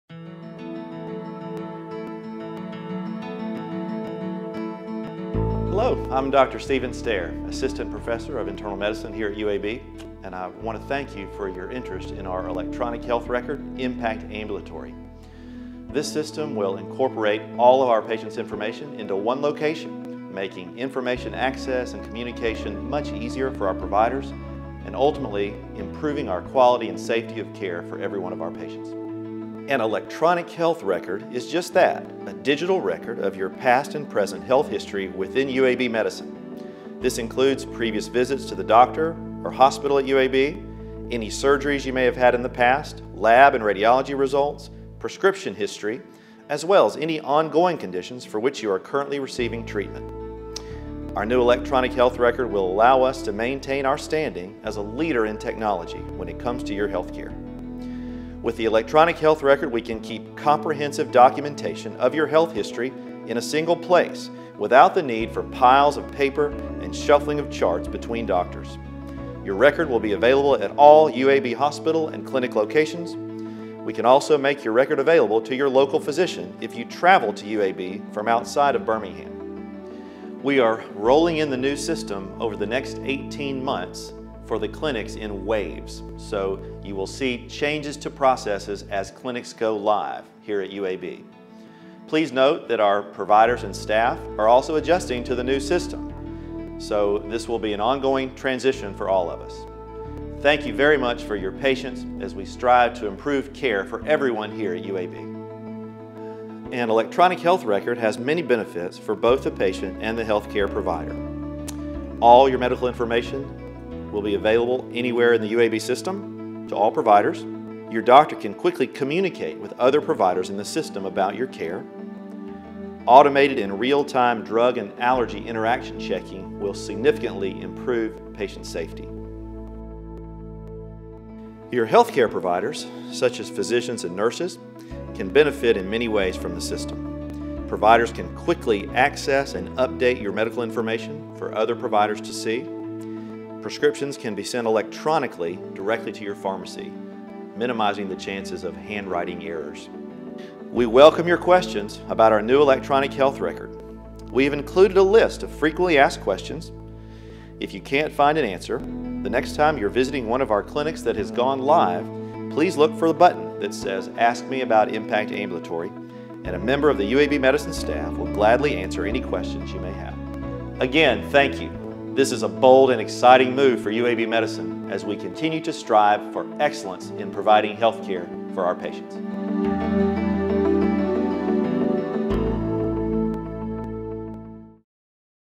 UAB Medicine Electronic Health Record - IMPACT Ambulatory
Stephen Stair, MD, FACP, the physician executive sponsor for UAB Medicine's Electronic Health Record, IMPACT Ambulatory discussing the first wave roll out of our new health record. Over the next 18 months, all UAB Medicine hospitals and clinics will be brought online in one of three waves.